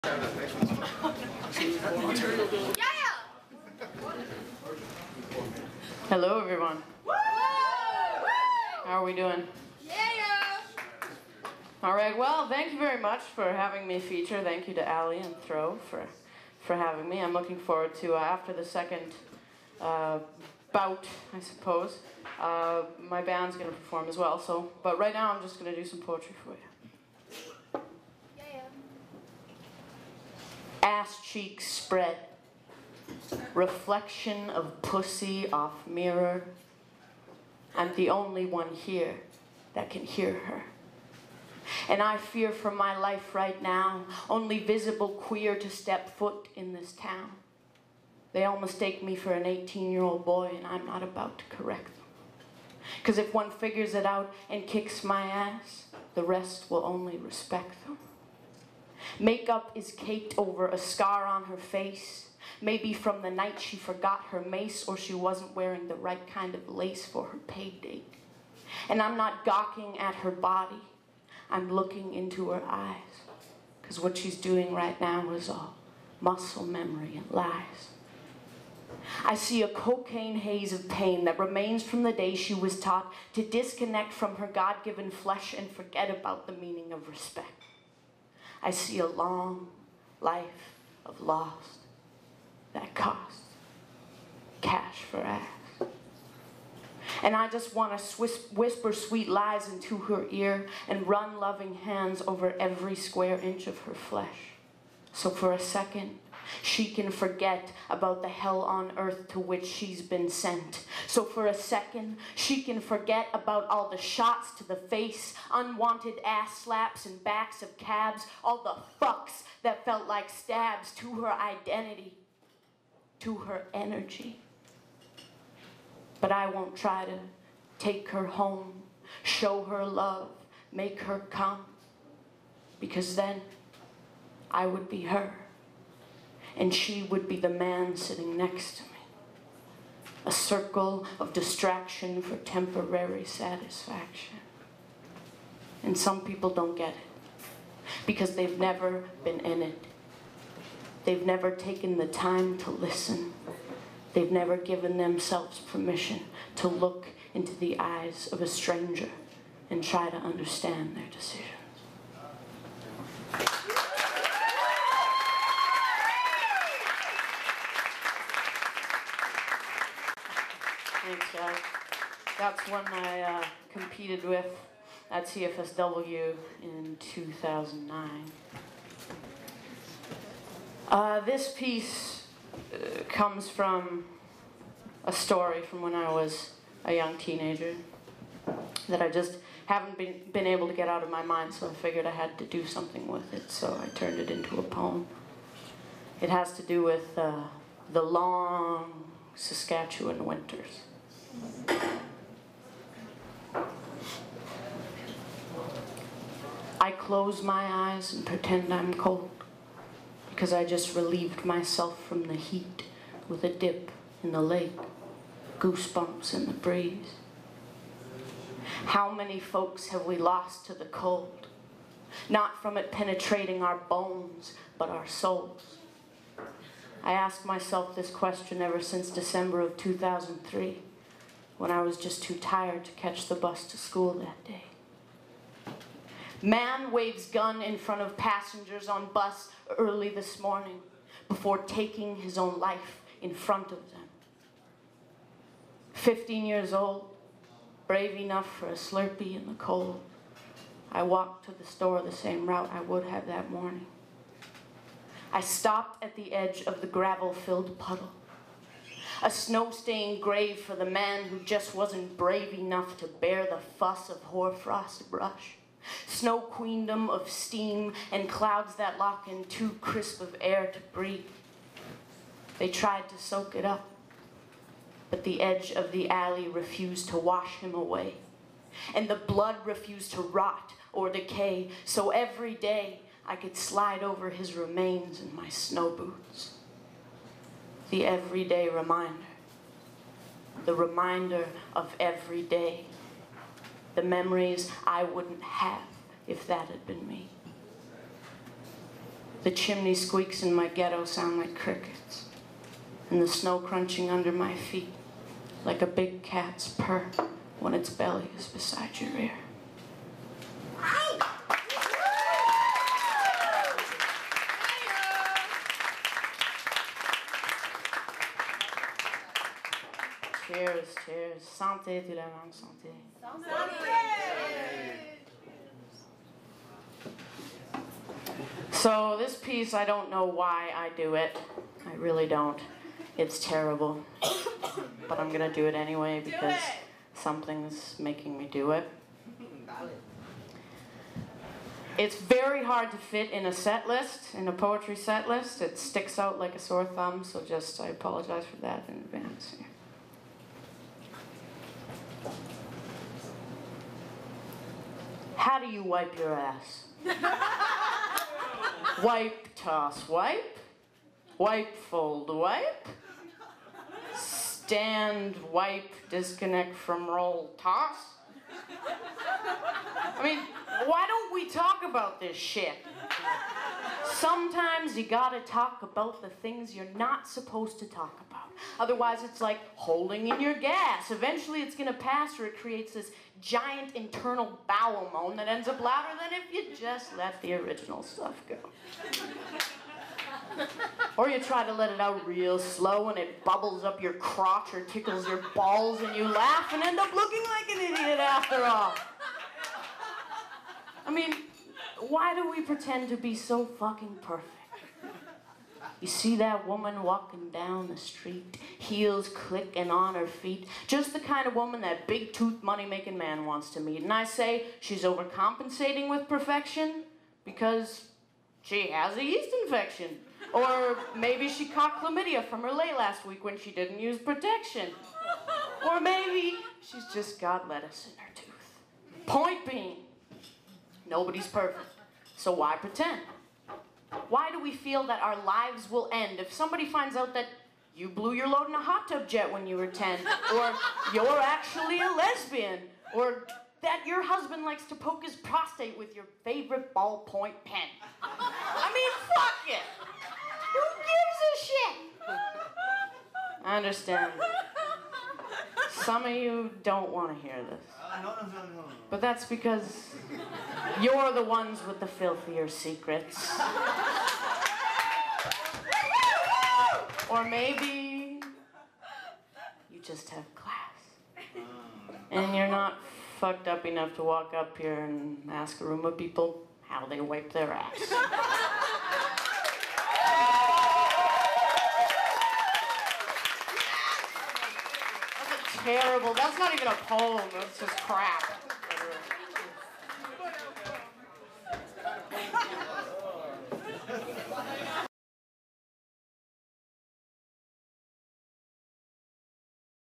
Megan Lane // Throw Poetry Slam, January 2011, Feature - Part 1
Megan Lane features at Throw's January Poetry Slam at Divan Orange in Montreal. The Throw Poetry Collective runs poetry slams at Divan Orange in Montreal, Québec on the last Sunday of every month.